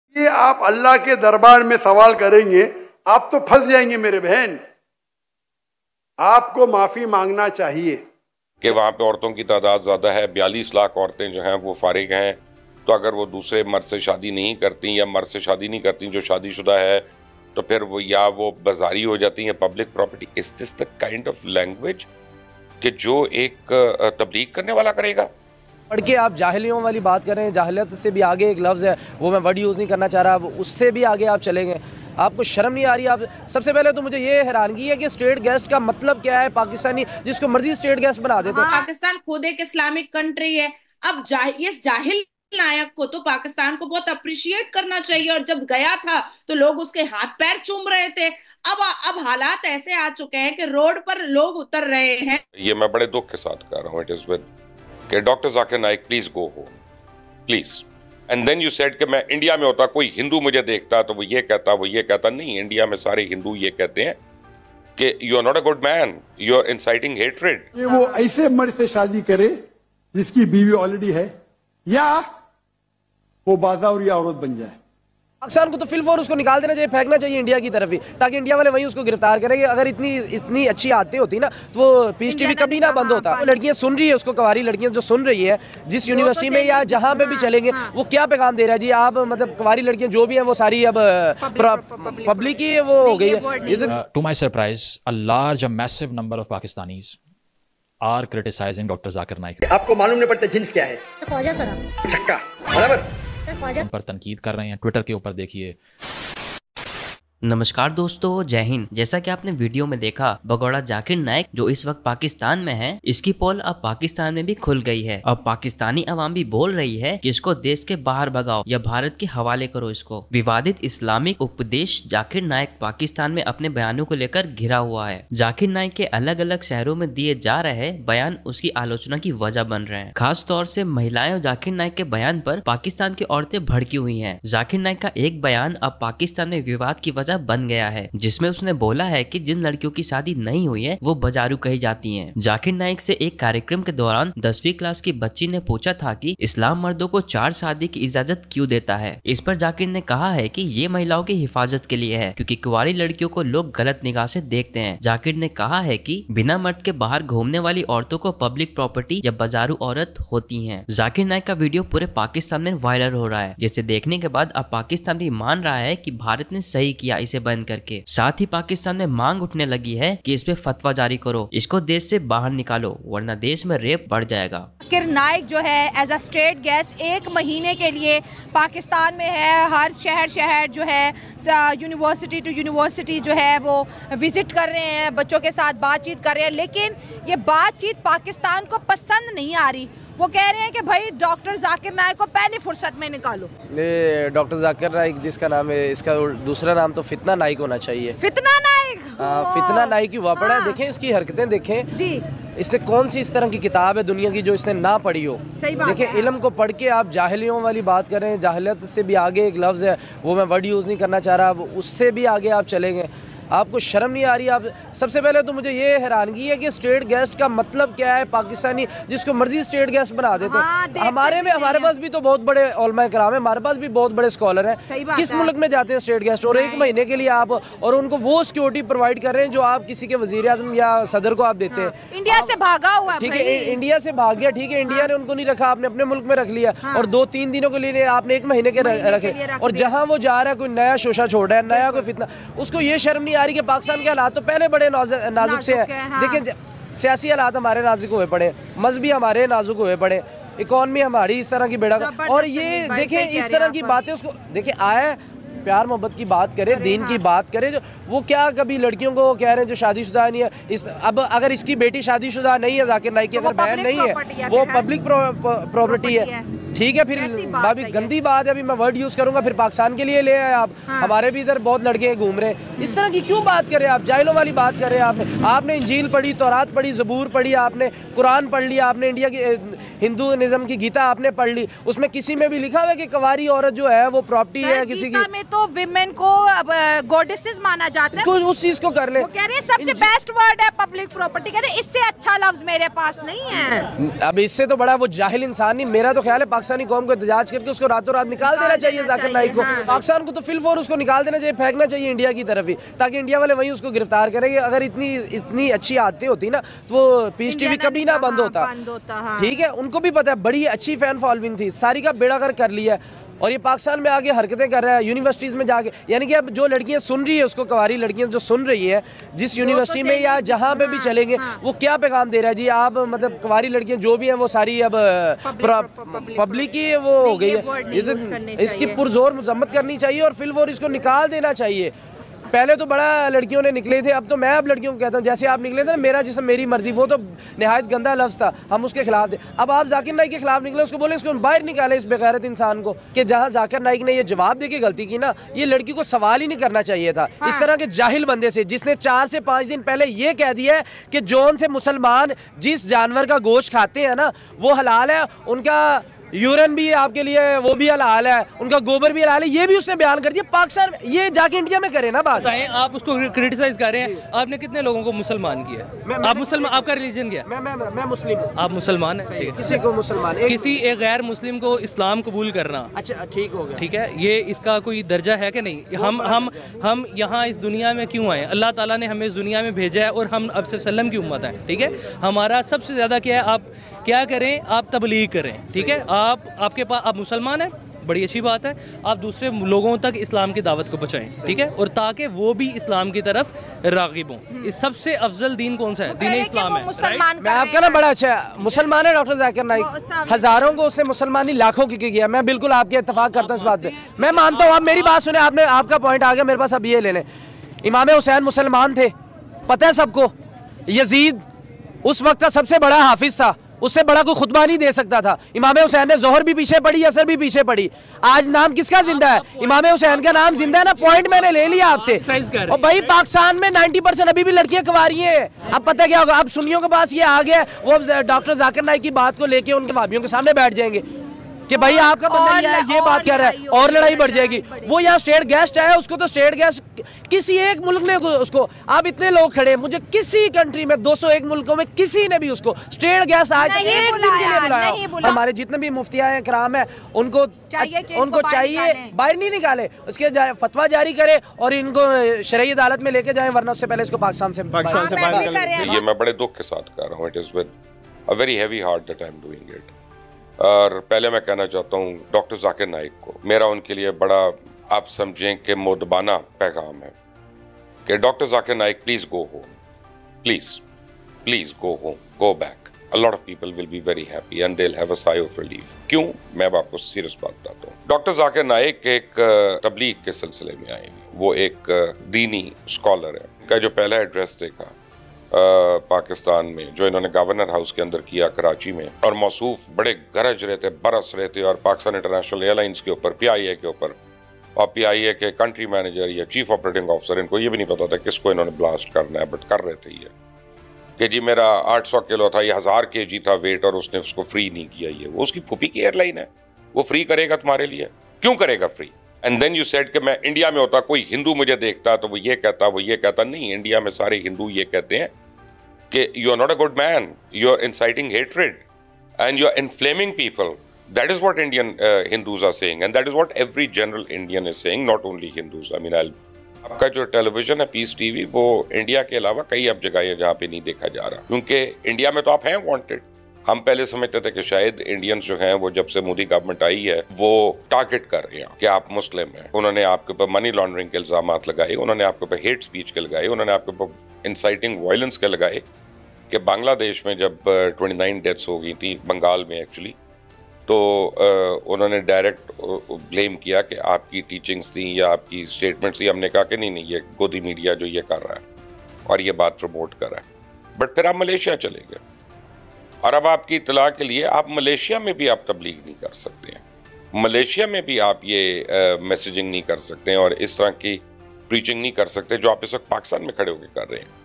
Bharat ke bhagode Zakir Naik ko bahar nikalo pak me molana bhide फतवा जारी पब्लिक ने लताड़ा
Dr. Zakir Naik on Pakistan